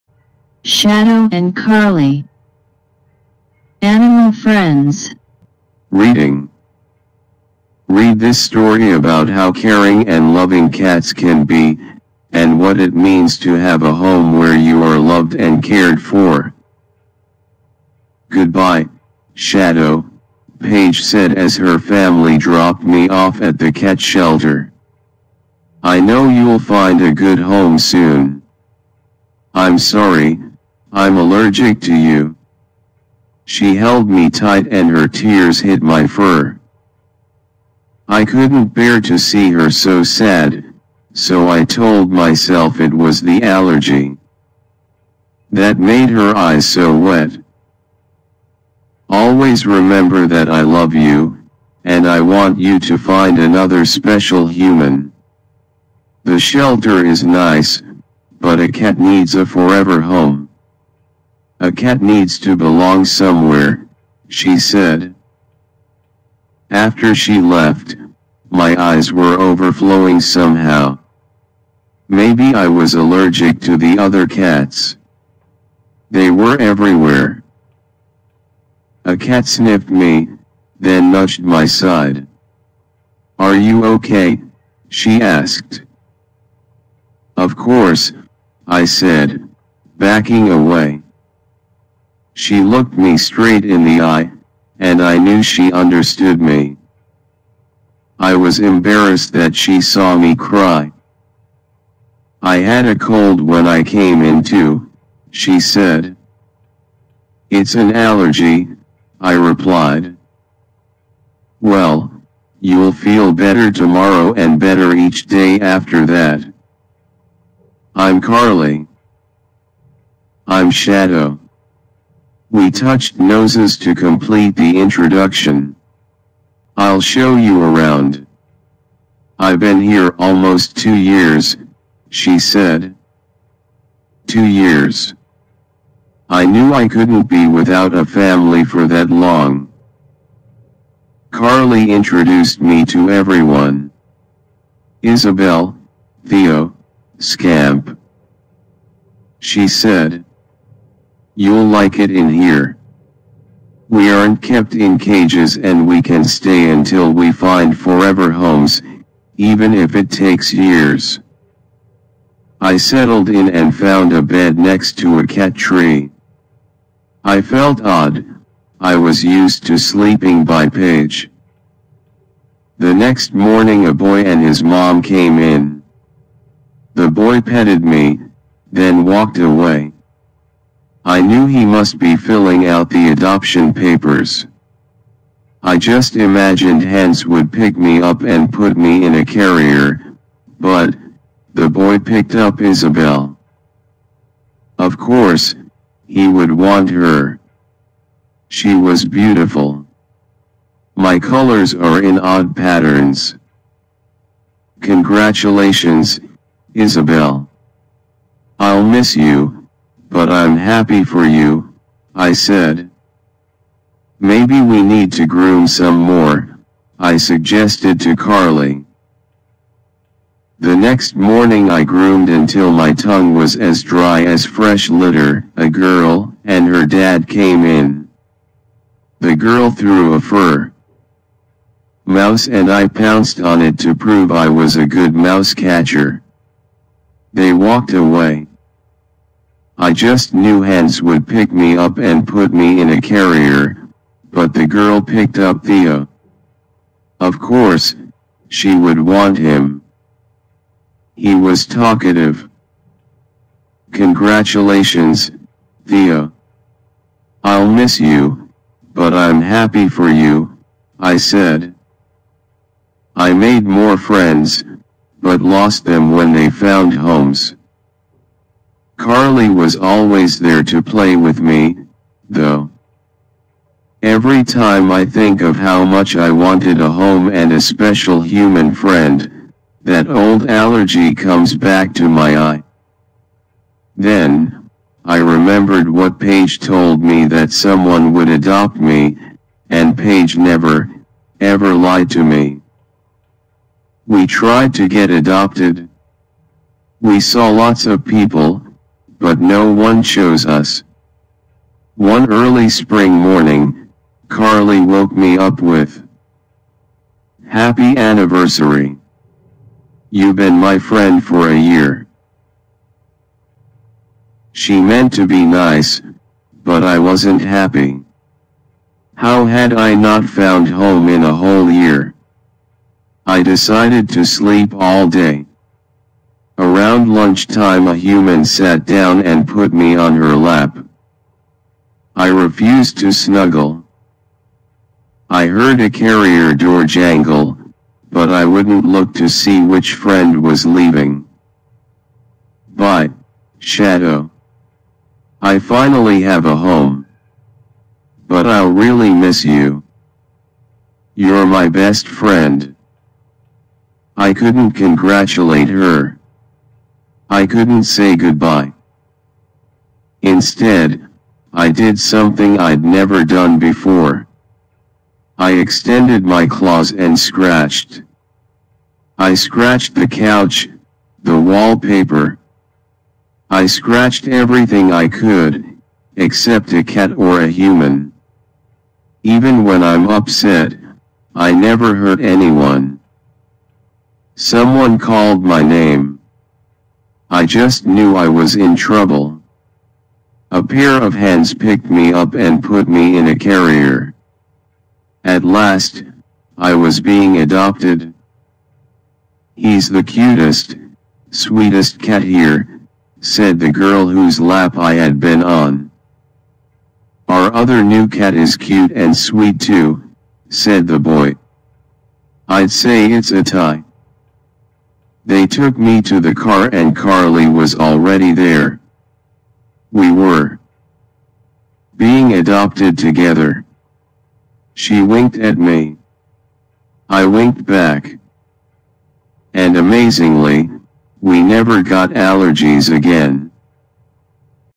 English Class 4 Shadow and Carly | Reading with accent
Learn the right pronunciation of English words present in this lesson.
Listen to the reading [with accent] of CBSE English Lesson 3 ' Shadow and Carly ' from English Alive CourseBook.
Listening to lessons in English accent will help students gain familiarity with the accent and interact with native English speakers with confidence and ease.